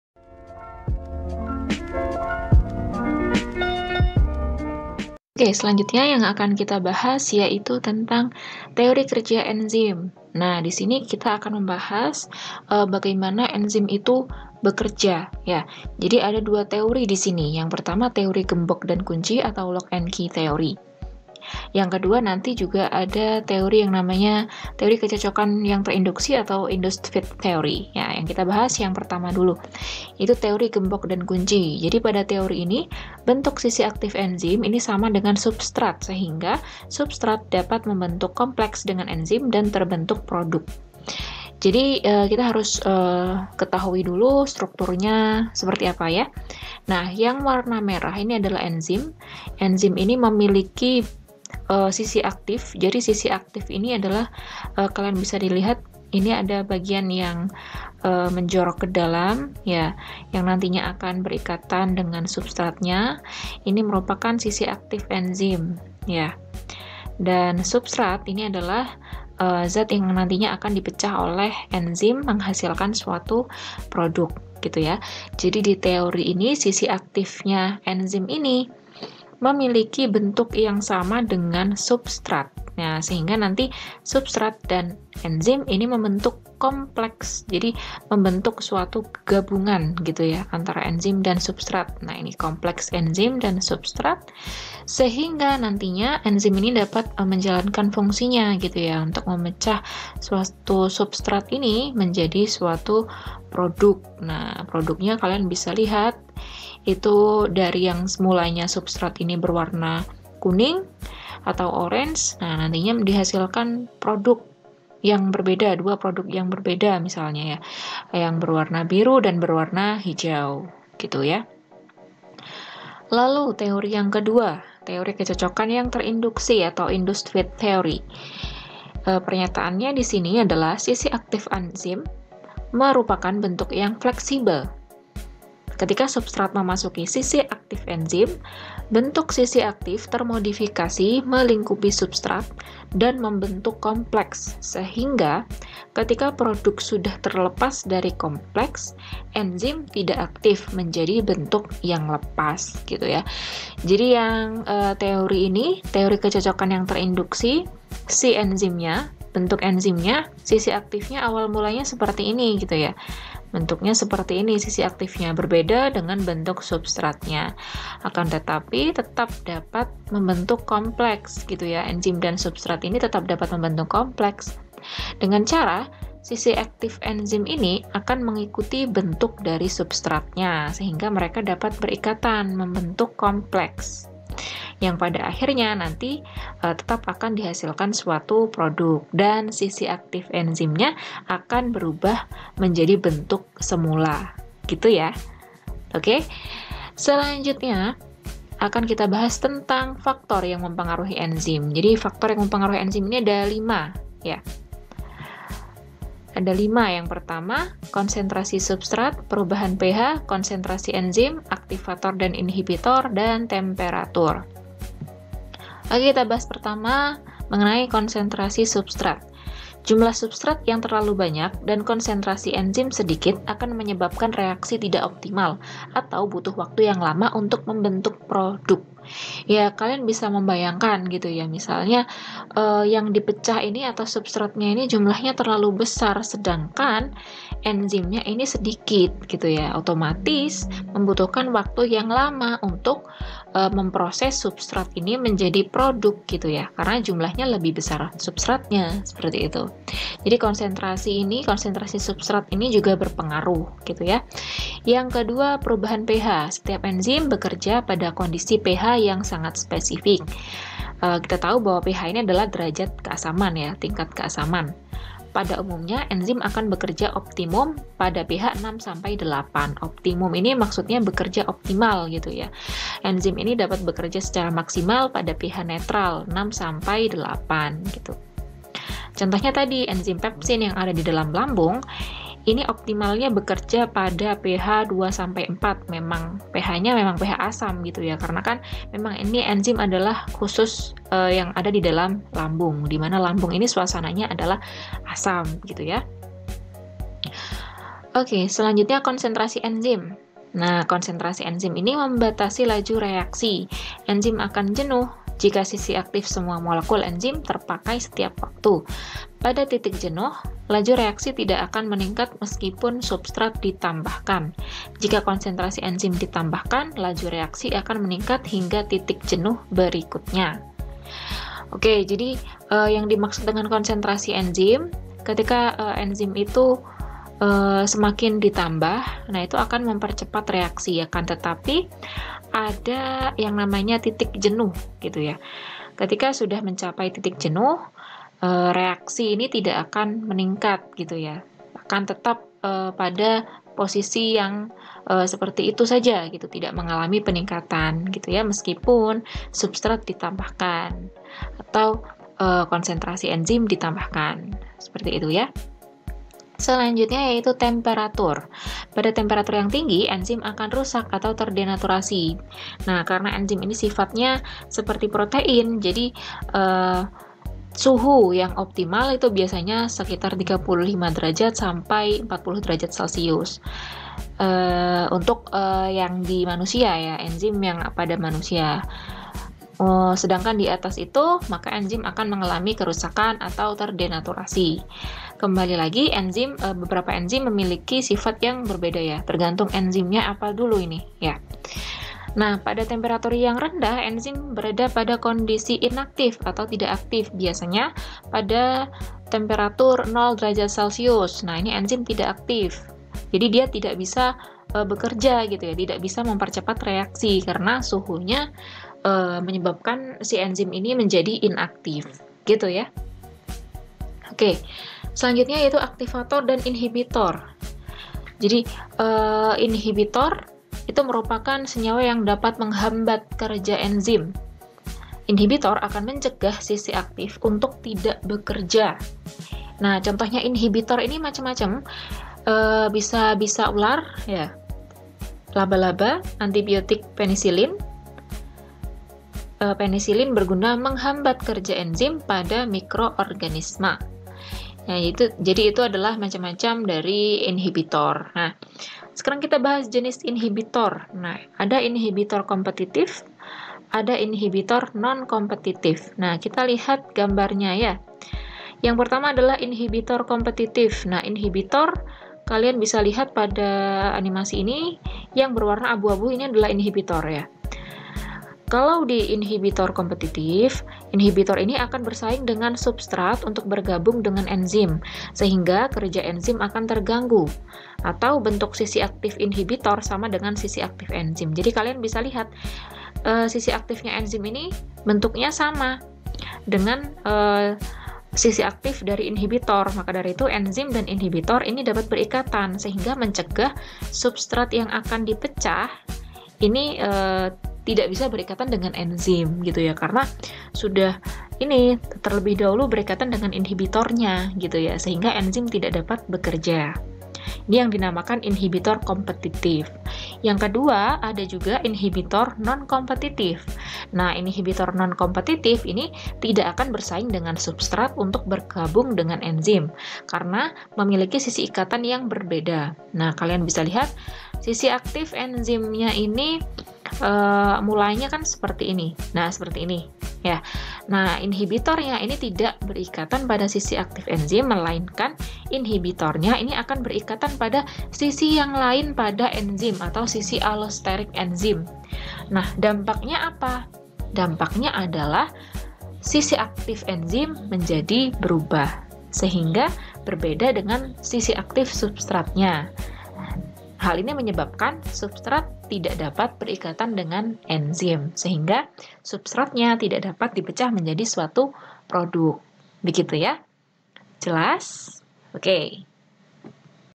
CARA KERJA DAN FAKTOR YANG MEMPENGARUHI ENZIM - BIOLOGI XII SMA/MA
Halo semuanya. Video kali ini saya membahas tentang materi selanjutnya di bab Metabolisme ini yaitu tentang Cara kerja enzim dan Faktor yang mempengaruhi kerja enzim.

Video ini saya buat menggunakan Multimedia Interaktif yang telah saya gunakan sebelumnya dalam melakukan pembelajaran di sekolah. Jika teman-teman ingin memiliki file multimedia interaktif ini agar bisa dipelajari secara offline, silahkan DM saya di akun instagram atau tulis email di kolom komentar agar saya dapat mengirim file ini agar lebih bermanfaat :)

Untuk kalian yang belum mempelajari materi sebelumnya tentang:

Pengertian enzim, metabolisme, struktur dan sifat enzim,
silahkan klik link di bawah ini: